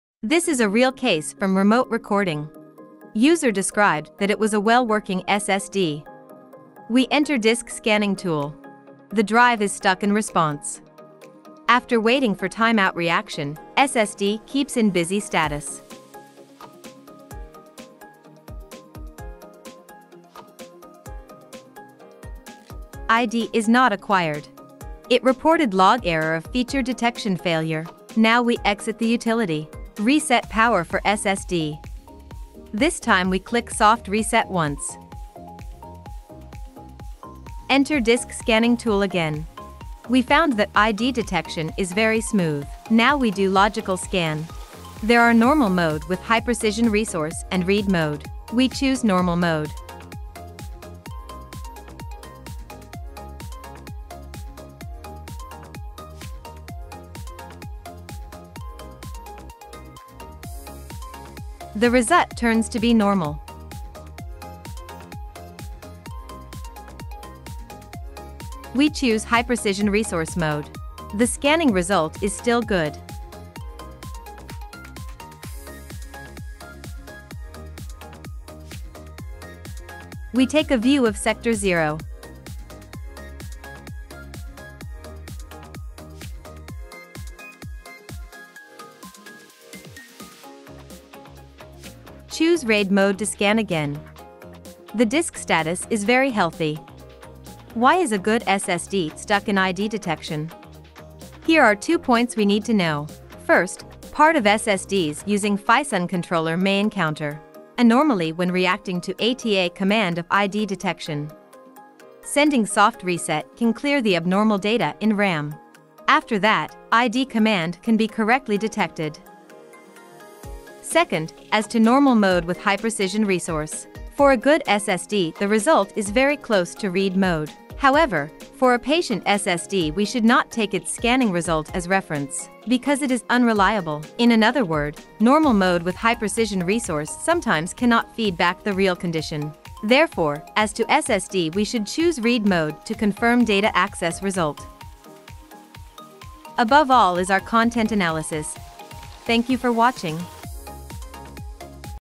Tricks for SSD ID Detection
According to the feedbacks from our users, we find that some SSDs may encounter ID detection anormaly even if it is a good drive
So the video below will show some tricks for SSD ID detection.